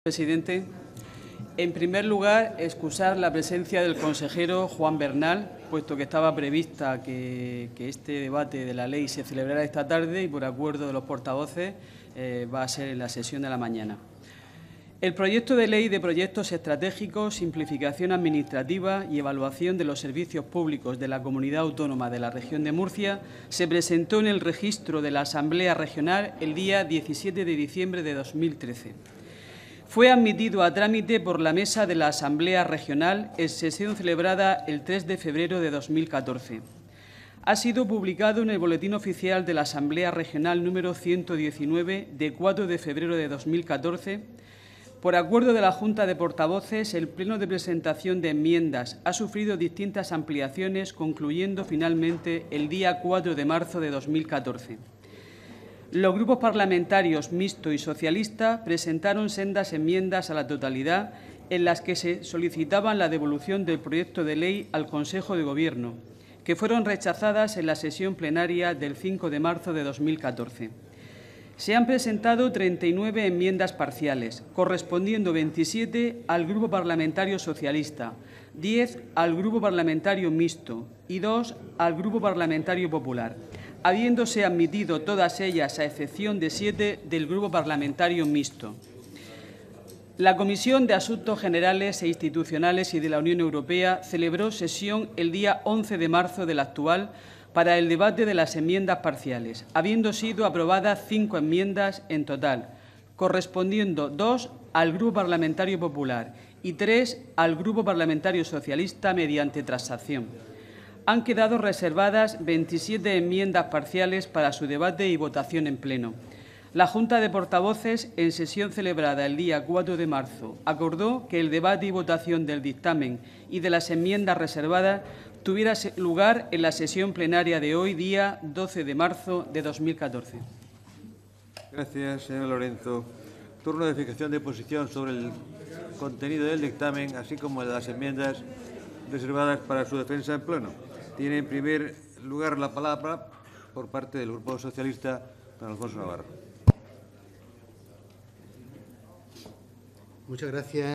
Lorenzo Gabarrón, Catalina. Dictamen de la Comisión de As. Grls. e Institucionales y de la U.E.
8L/PL-0019 DEBATE Y VOTACIÓN DEL DICTAMEN DE LA COMISIÓN DE ASUNTOS GENERALES E INSTITUCIONALES Y DE LA UNIÓN EUROPEA AL PROYECTO DE LEY DE PROYECTOS ESTRATEGICOS, SIMPLIFICACION ADMINISTRATIVA Y EVALUACION DE LOS SERVICIOS PUBLICOS DE LA COMUNIDAD AUTONOMA DE LA REGION DE MURCIA.